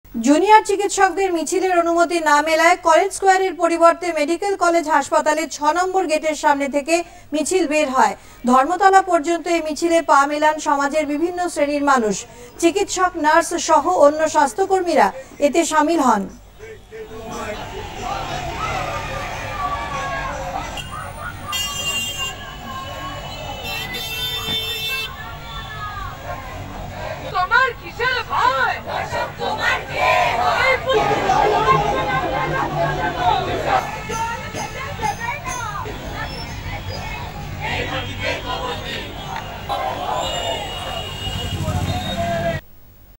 মেডিক্যাল কলেজ হাসপাতালের ৬ নম্বর গেট থেকে মিছিল
জুনিয়র চিকিৎসকদের মিছিলের অনুমতি না মেলায় কলেজ স্কোয়ারের পরিবর্তে মেডিক্যাল কলেজ হাসপাতালের ৬ নম্বর গেটের সামনে থেকে মিছিল বের হয়